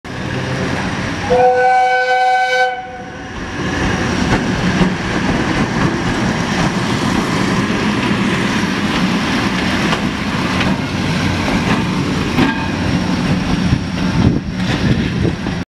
Japan Electric Railway Locomotive
A move an a Japanese box motor electric locomotive on the Chichibu Railway.